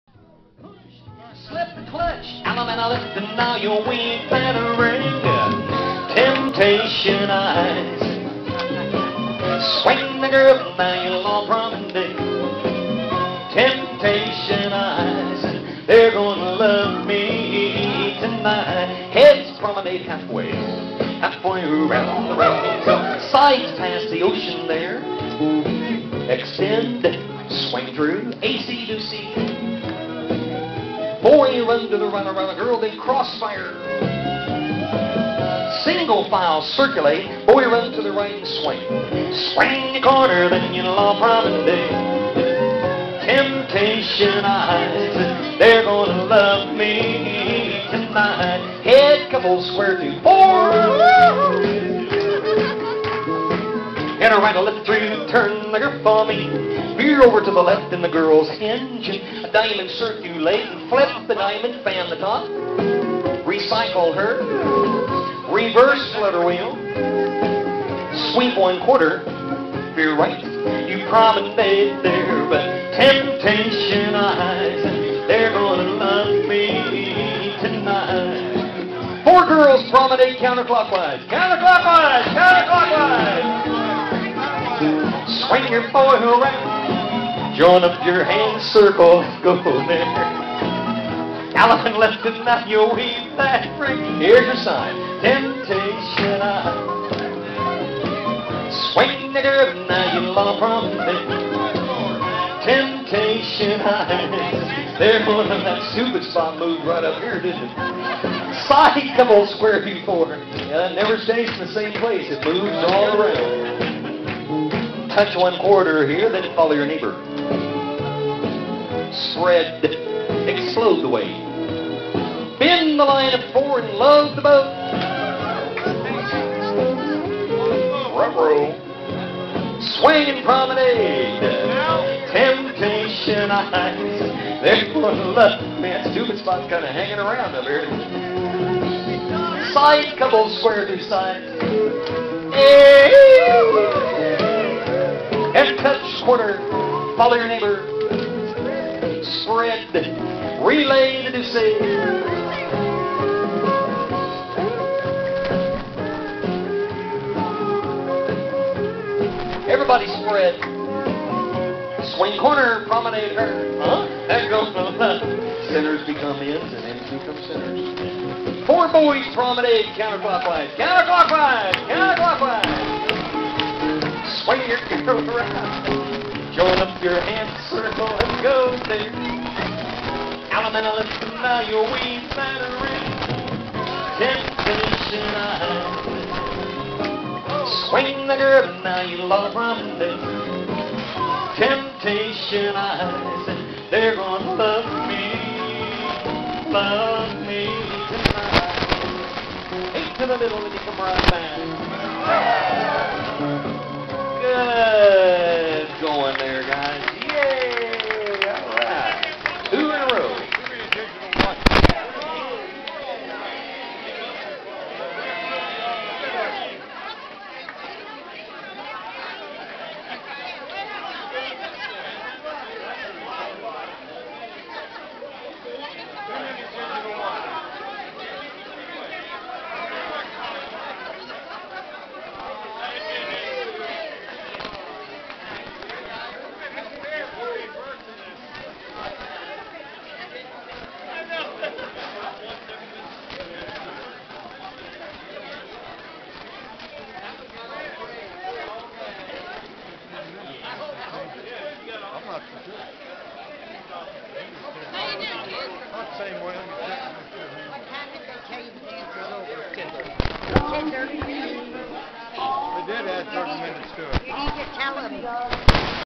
Square Dancing in Gulfport, Mississippi with Tom Roper square dance caller PICT0004.ASF
Tom Roper in Gulfport, Mississippi on July 30, 2011